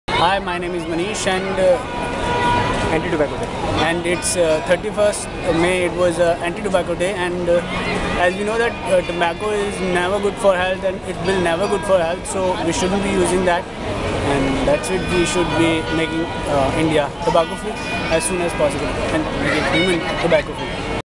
Quit Smoking | World No Tobacco Day | Manish
As we all know that Smoking is injurious to health and Tobacco has done no good to anyone, Public Voice India celebrates World No Tobacco Day to spread awareness of the risks of Tobacco consumption. Share your opinion on what you  think on Tobacco consumption. Watch Quit Smoking | World No Tobacco Day | Manish to make yourself aware on how society is directly or indirectly suffering the adverse effects of tobacco.
Also, comment and send your feedback on public voice channel videos.